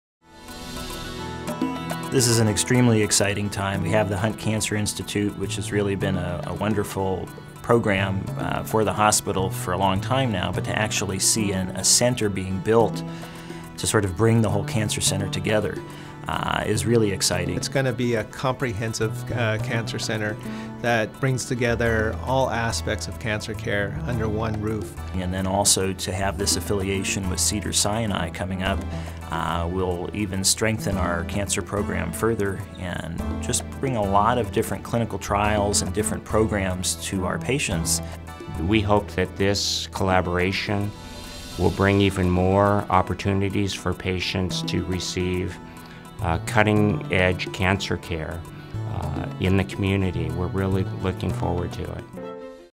Hunt Cancer Center - A New Hope for the South Bay
In Fall 2019, Torrance Memorial will open the new Hunt Cancer Center, a 37,000-square-foot facility, designed to consolidate and strengthen cancer care in the South Bay.